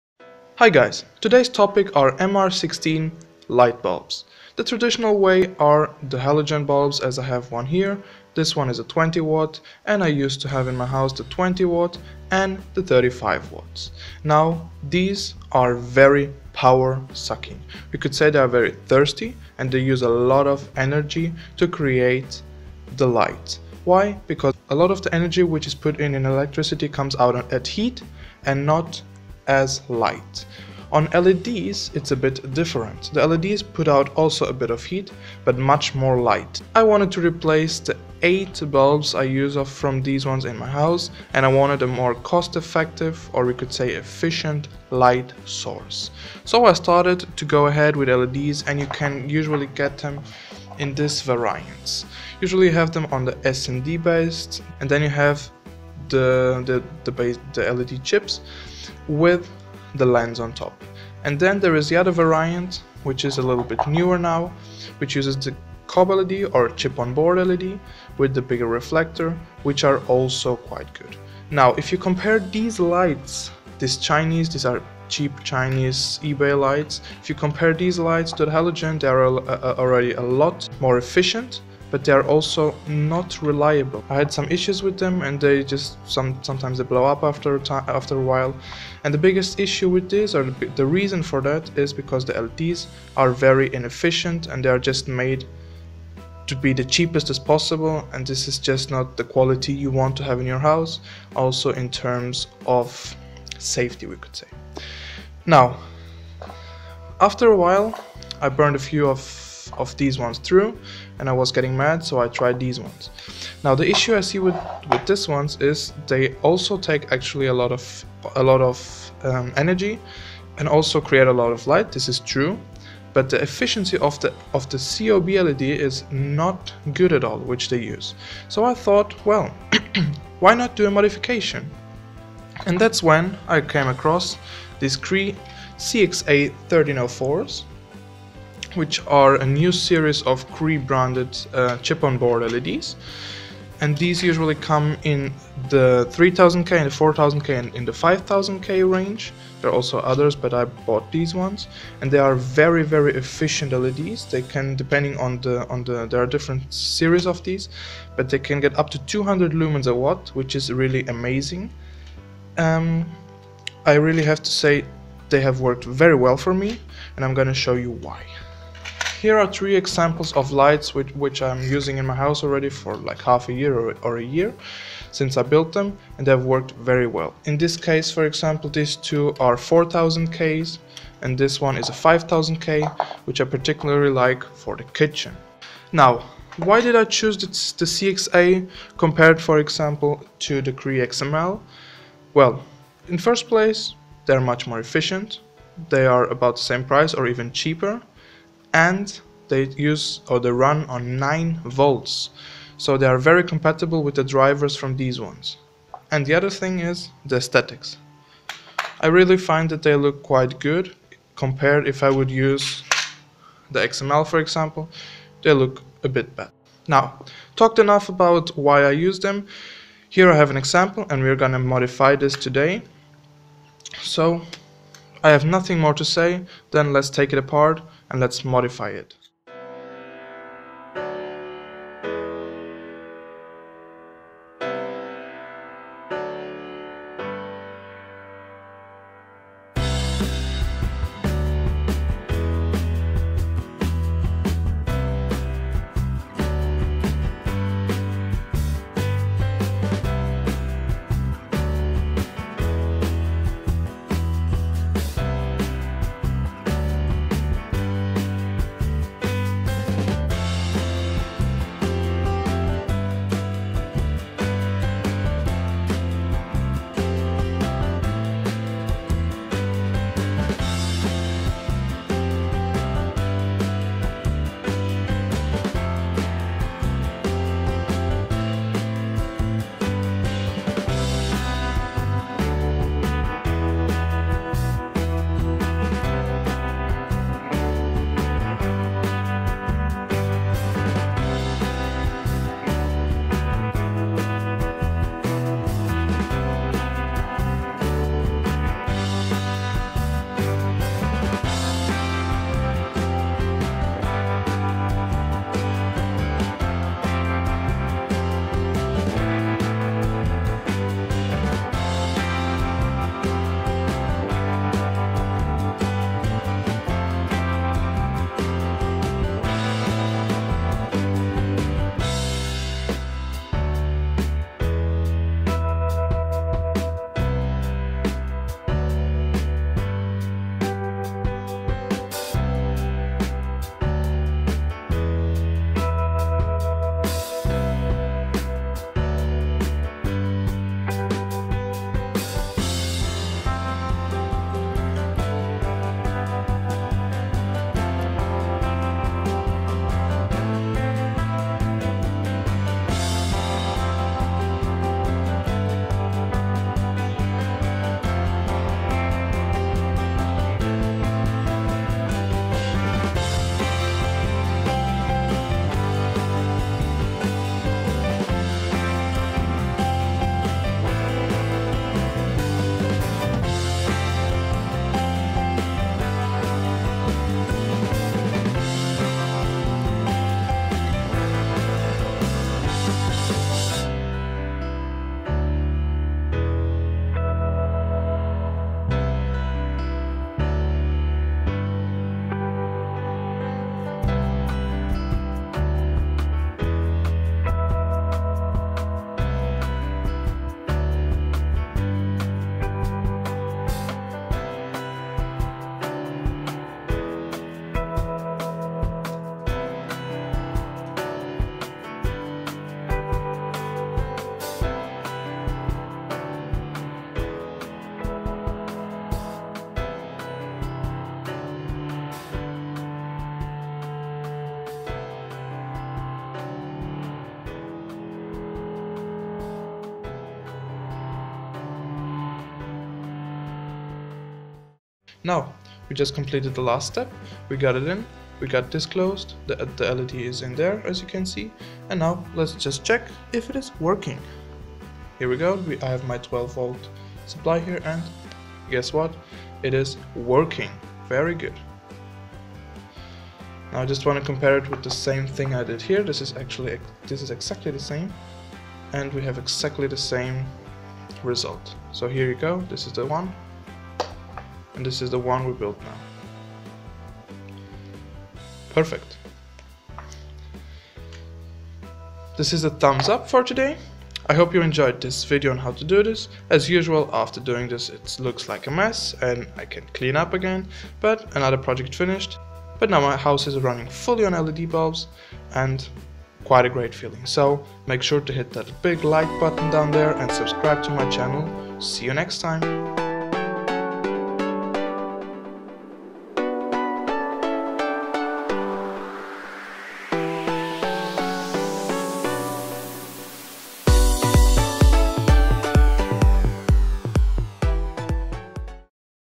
MOST EFICIENT DIY MR16 bulb, modification to cree Cxa1304 (How to)
I get about 100% increase in eficiency compared to the original COB led bulb.
Modifying these cheaply built light with quality Cree`s make them really reliable, eficient and cheap!
material list:
DRIVER: MR16 3x1w/3x3w led driver(deppending on your current preference), I used the 3x1w one. example eBay item number: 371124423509
LED: 9v Cree CXA1304 (color temp is your choice, make sure to get the 9v version) example: eBay item number: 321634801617

Try it out yourself!

PS: to hold the driver in the base I just applied some hot-glue(black in this case)